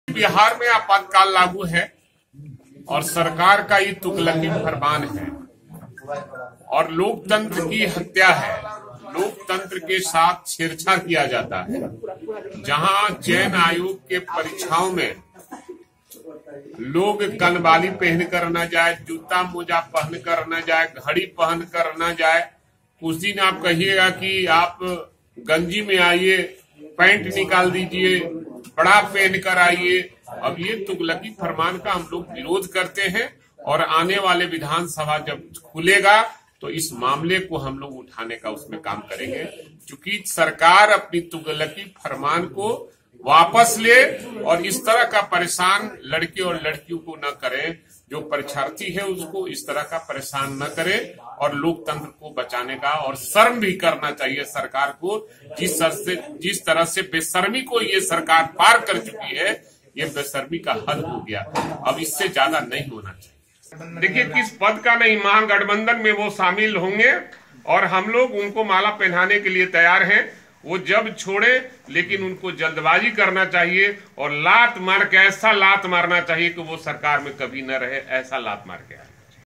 RJD ने CM नीतीश पर बोला जम के हमला.. देखिए video | News4Nation
राजद ने सीएम नीतीश पर जम के हमला बोला है , राजद विधायक भाई वीरेंद्र ने क्या कुछ कहा है देखिए ...